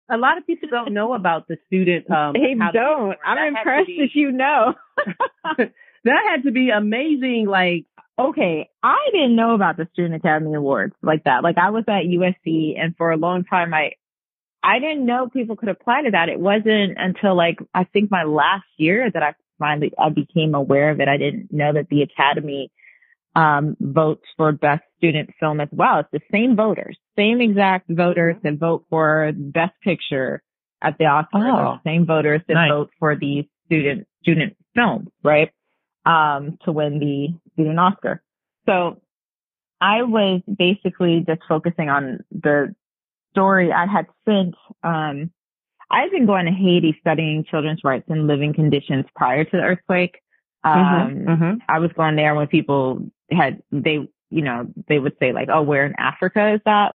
The Storytelling Journey of Award-winning Filmmaker Kelley Kali - 60s
In this latest episode of 'Talkin Smack Intellectually' featuring the incredibly talented Kelley Kali! An LA native and powerhouse in the film industry, Kelley's journey from Howard University to student Academy Award-winning director, writer, and producer is truly inspiring.

 In this episode, we delve into Kelley's cinematic journey and her groundbreaking project 'KEMBA,' now available to watch on  BET Original. Kelley shares her experiences spanning countries like Belize, Haiti, China, and the USA, discussing her mission to amplify voices within marginalized communities through storytelling. Hear firsthand about her latest projects and the impact of 'KEMBA' in shedding light on systemic issues and the fight for justice.

We also explore her award-winning film "LALO’S HOUSE" that won the 45th Annual Student Academy Award. Discover how these achievements have shaped her future projects and aspirations in the film industry. Kelley dives into the inspiration behind telling Kemba Smith's story, highlighting resilience, hope, and the pursuit of justice and freedom. She aims to shed light on the complexities of the 'war on drugs,' unfair sentencing practices, and the importance of second chances and redemption through her work. As a director and content creator, Kelley's storytelling has been recognized by prestigious organizations and film festivals, focusing on important issues faced by marginalized communities.

 She shares her goals and aspirations for the future, aiming to raise awareness and foster positive change through her impactful narratives. For aspiring women in the film industry, Kelley offers guidance on navigating their path to success, drawing from her own experiences and challenges she has overcome along her remarkable journey.

Check out the latest episode of Talkin’ Smack, Intellectually!!